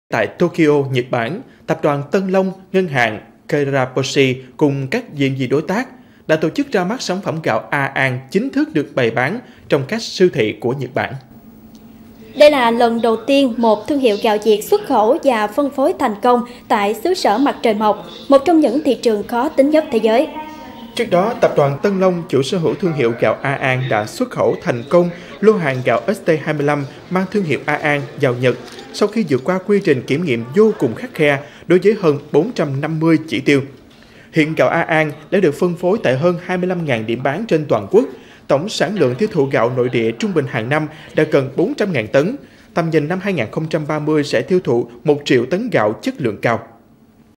Gạo A An chinh phục thị trường Nhật Bản | Cần Thơ TV
Mới đây tại Tokyo, Nhật Bản, Tập đoàn Tân Long, ngân hàng Kiraboshi cùng các đơn vị đối tác đã tổ chức ra mắt sản phẩm gạo A An chính thức được bày bán trong các hệ thống siêu thị của Nhật Bản.